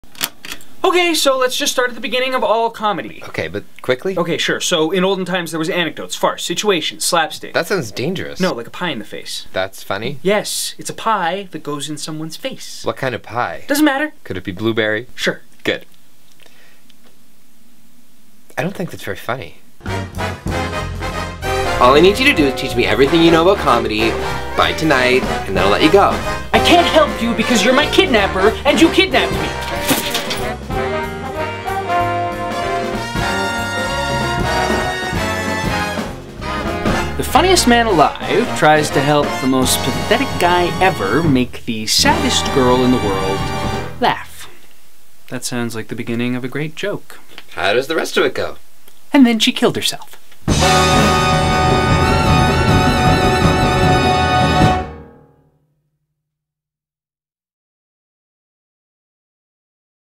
Theatre Brouhaha's PUNCH UP - Trailer
The Funniest Man Alive tries to help the Most Pathetic Man That Ever Existed make the Saddest Girl in the World laugh. Toronto's hottest young playwright comes to Cape Breton with this darkly comic love letter to comedy.

WRITTEN BY
Kat Sandler

DIRECTED BY
Kristen Gregor

FEATURING:
Nicholas Porteous
Wesley J. Colford
Hilary Scott

PRAISE FOR THE 2014 TORONTO FRINGE PRODUCTION:
*Best of Fringe*
*Outstanding New Play*
*Outstanding Production*
*Patron's Pick*

"Theatre Brouhaha returns to the Fringe in full force with Punch Up, a rapid-fire black comedy from the always brilliant Kat Sandler."
- The Torontoist

Live Music after every show - cover included in ticket price:

Feb. 11 - Carmen Townsend
Feb. 12 - Danny MacNeil & Colette Deveaux
Feb. 13 - The Novemberists
Feb. 14 - The Jaynes
Feb. 15 - John Gill